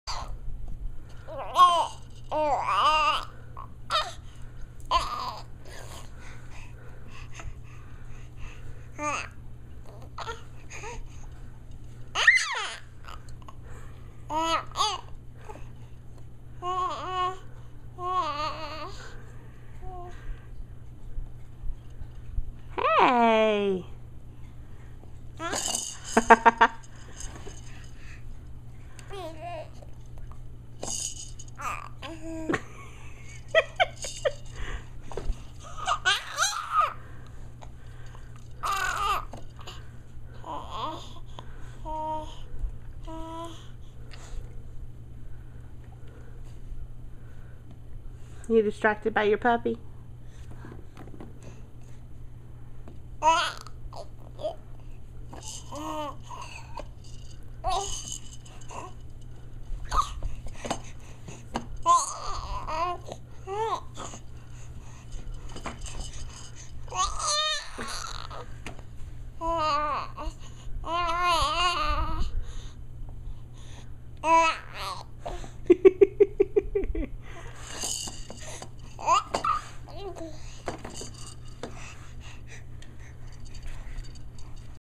Abby found her squeaker
Abby Reid found her voice today...her squeal!  Oh joy! :)